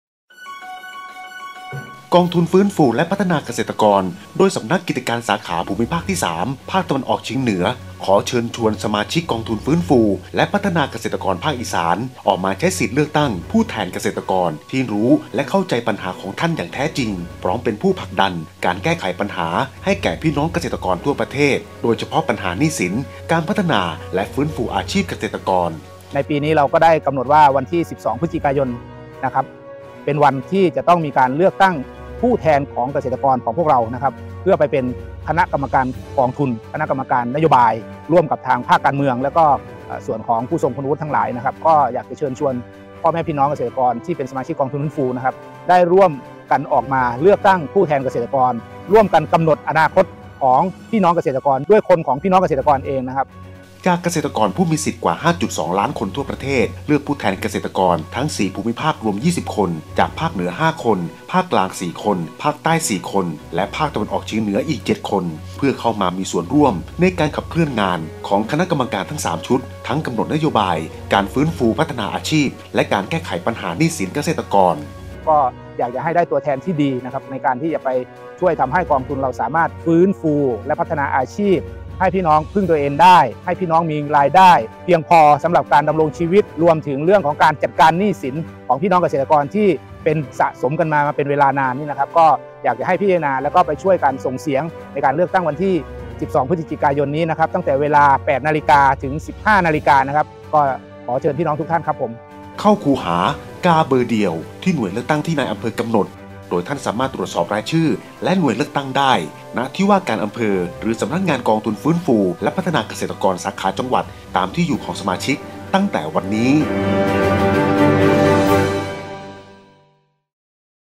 กองทุนฟื้นฟูและพัฒนาเกษตร เชิญชวนสมาชิกกองทุนฟื้นฟูฯ ภาคอีสาน เลือกตั้งผู้แทนเกษตรกร
กองทุนฟื้นฟูและพัฒนาเกษตรกร โดย สำนักกิจการสาขาภูมิภาคที่ 3 ภาคตะวันออกเฉียงเหนือ  จึงขอเชิญชวนสมาชิกกองทุนฟื้นฟูและพัฒนาเกษตรกรภาคอีสาน ออกมาใช้สิทธิ์เลือกตั้ง ผู้แทนเกษตรกร ที่เข้าใจปัญหาของท่านอย่างแท้จริง พร้อมเร่งผลักดันและแก้ไขปัญหาให้แก่พี่น้องเกษตรกรทั่วประเทศ โดยเฉพาะปัญหาหนี้สิน การพัฒนาและฟื้นฟูอาชีพเกษตรกร
     ในวันอาทิตย์ที่ 12 พฤศจิกายน 2566 ในเวลา 08.00 น. - 15.00 น. เข้าคูหากาเบอร์เดียว  ที่หน่วยเลือกตั้งที่นายอำเภอกำหนด โดยท่านสามารถใช้บัตรประจำตัวประชาชนตรวจสอบรายชื่อและหน่วยเลือกตั้งได้ ณ ที่ว่าการอำเภอ หรือ สำนักงานกองทุนฟื้นฟูและพัฒนาเกษตรกรสาขาจังหวัดตามที่อยู่ของสมาชิกได้แล้ว ตั้งแต่วันนี้